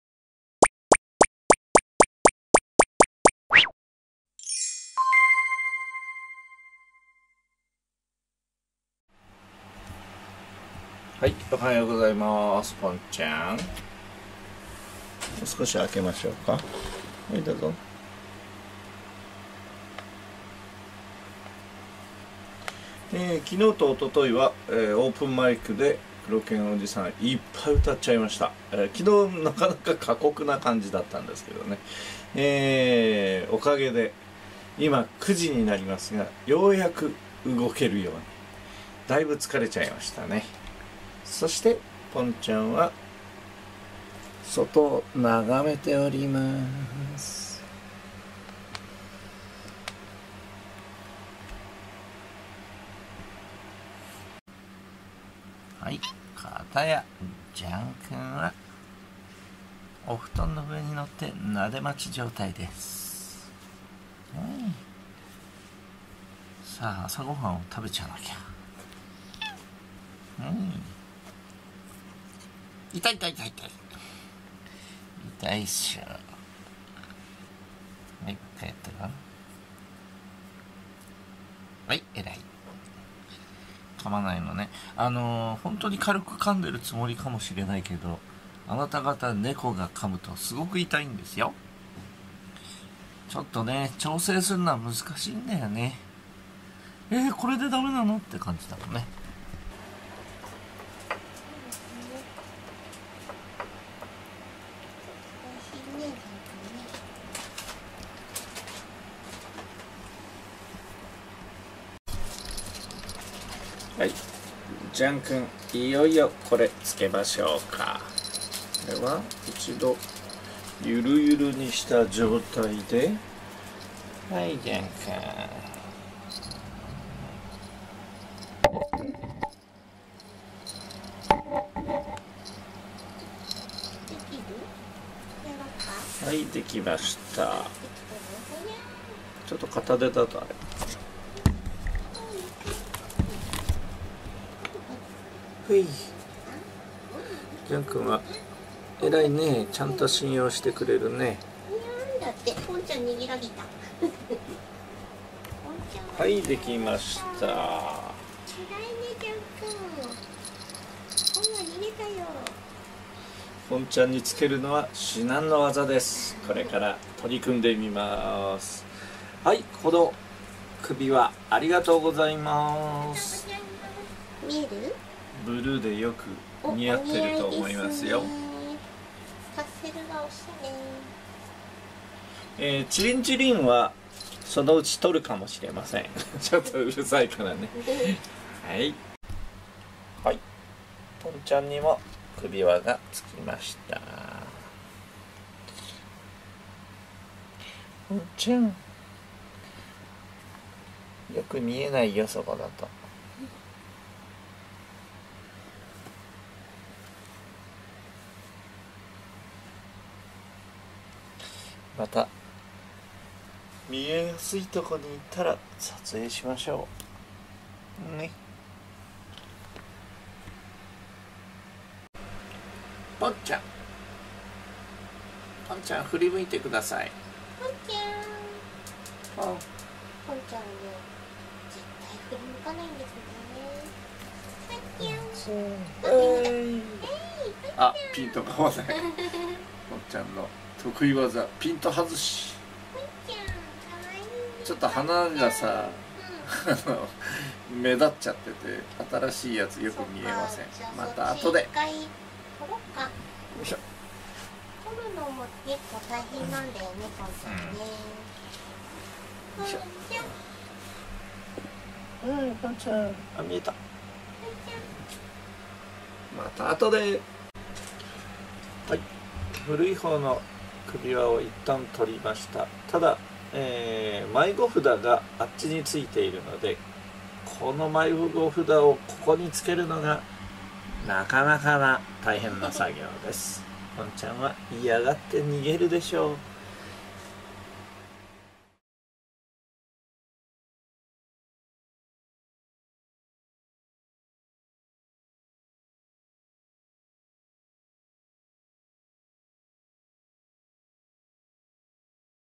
ジャンくんポンちゃんにいただきものの首輪を装着！【Jean & Pont 1909】2019/10/20
ジャンくんには水色、ポンちゃんにはピンクの紐で編んだ首輪をいただいたので付け替えました。
今まで使っていたものが少しくたびれてきていたのでとても嬉しいです。
色もよく似合うものを選んでもらってよく似合うでしょ？